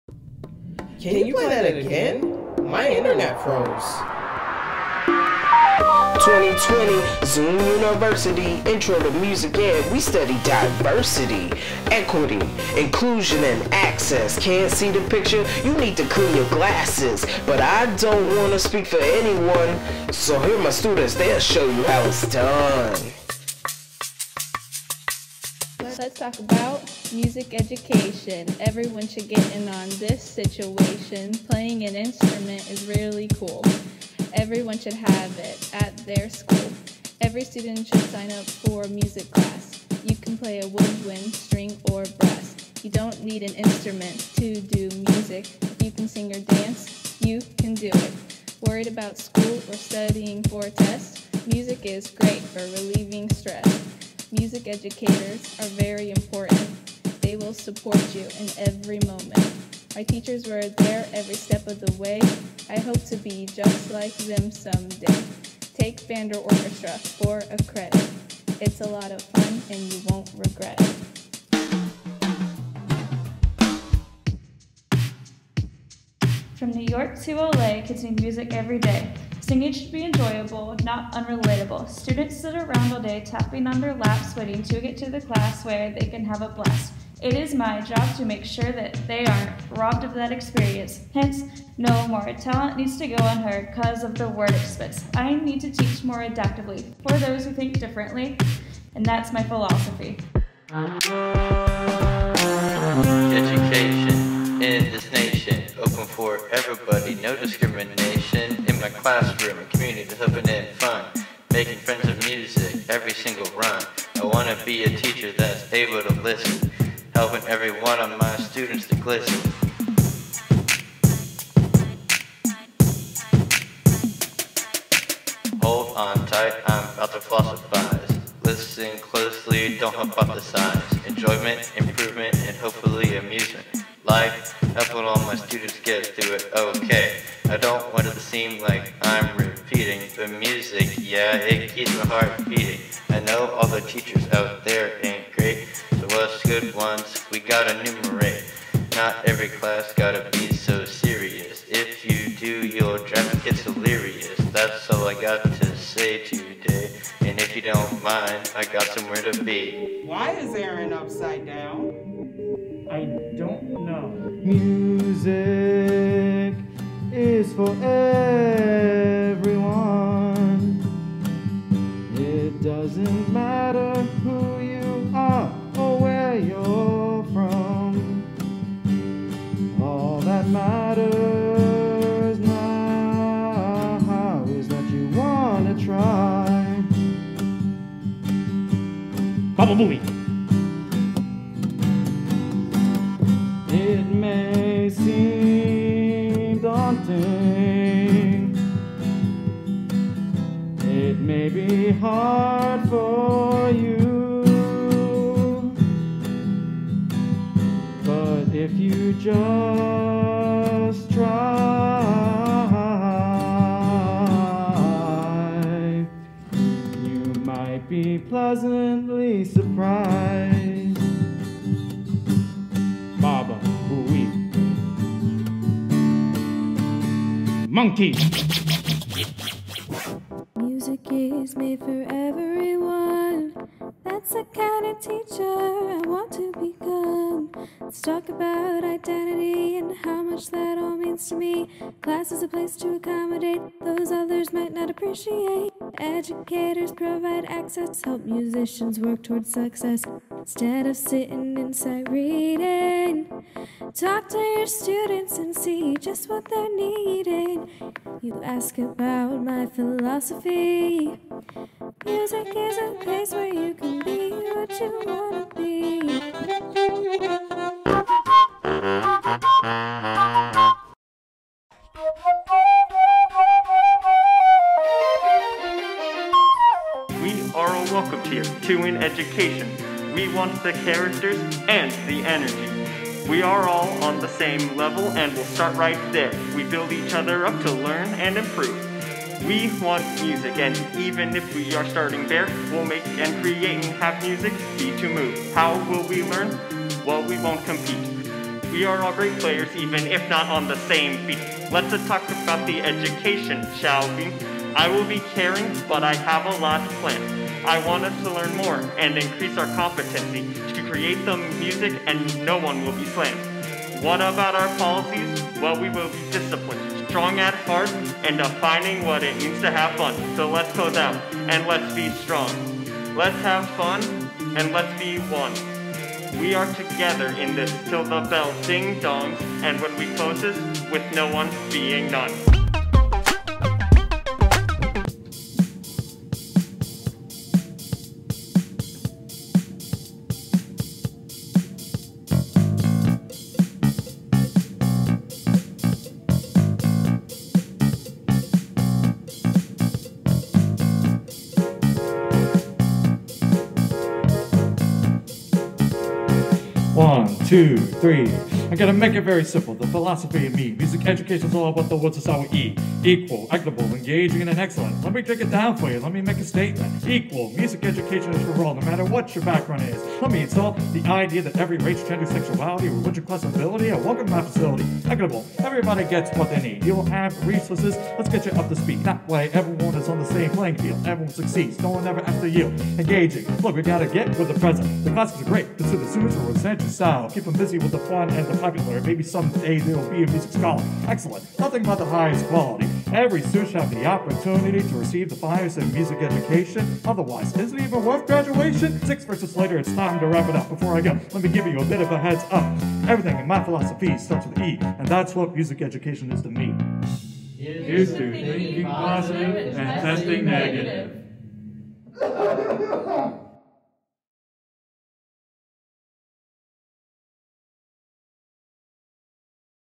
Philosophy of Music Education Song F20
Welcome to the [first full] year of Covid-19. We had to be socially distanced and masked, but we still managed to make music and further discover what kind of music educators we would like to be.